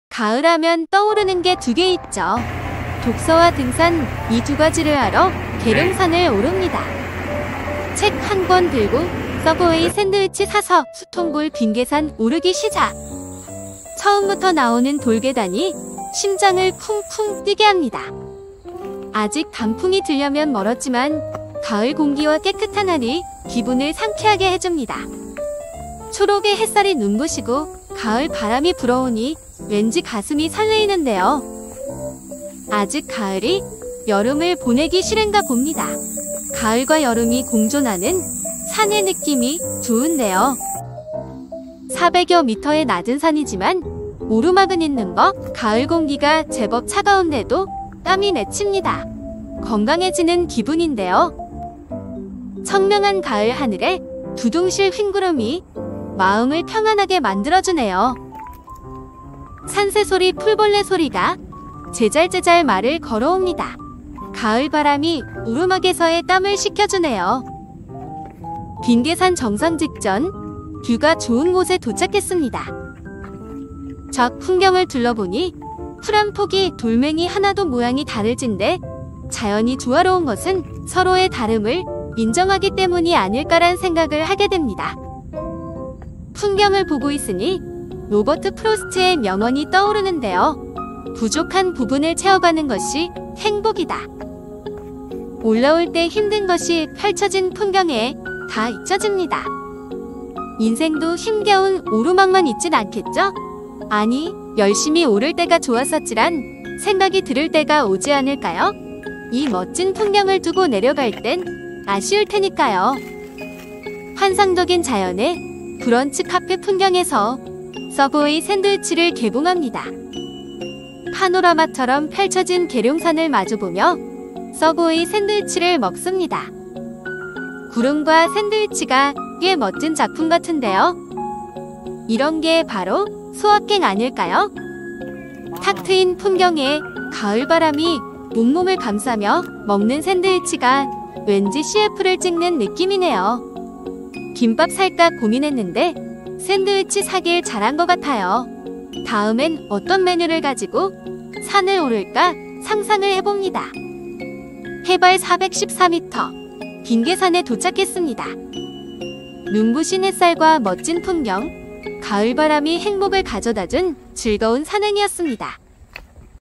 계룡산에서 독서와 써브웨이 샌드위치 먹으며 소확행.
독서의 계절 가을에 계룡산에 올라 책도 읽고 샌드위치 먹으며 소확행 했습니다.